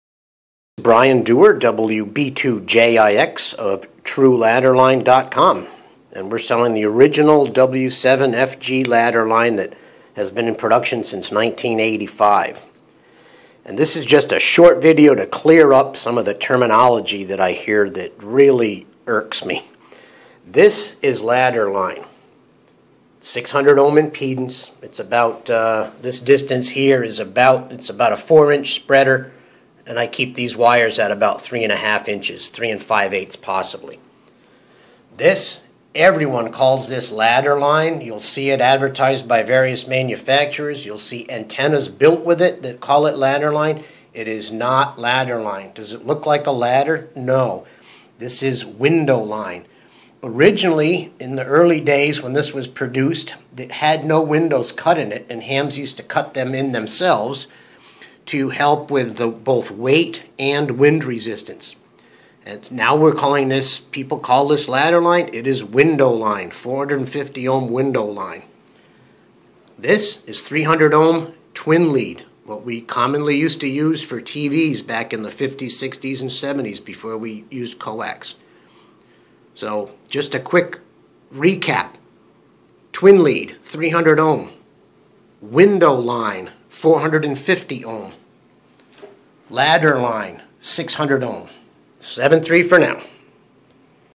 W7FG True Ladder Line and Doublets Ladder Line Window Line Twinlead?
Is it called Ladder Line, Window Line or Twinlead?